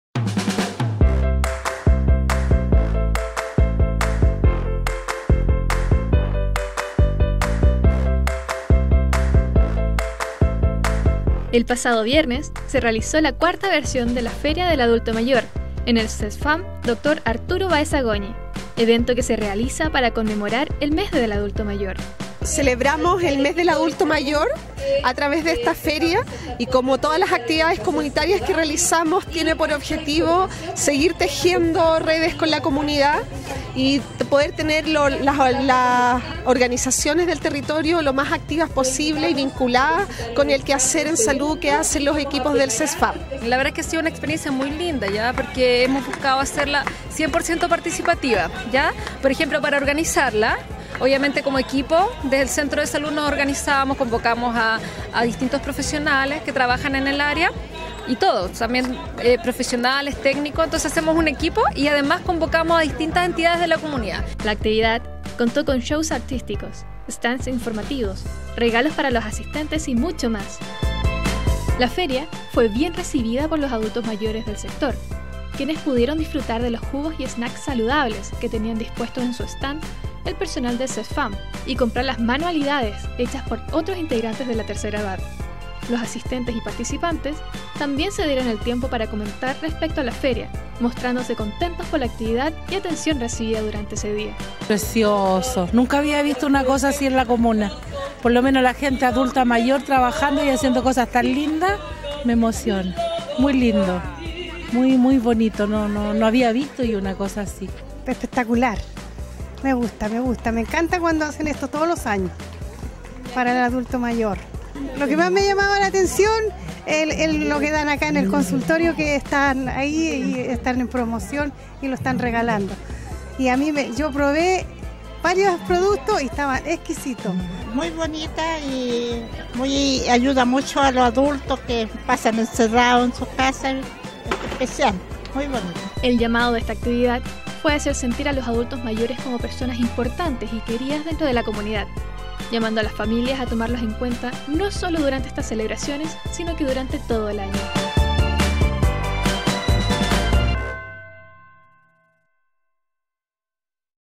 Feria del Adulto Mayor
El pasado viernes se celebró la cuarta versión de la Feria del Adulto Mayor en el Cesfam Dr. Arturo Baeza Goñi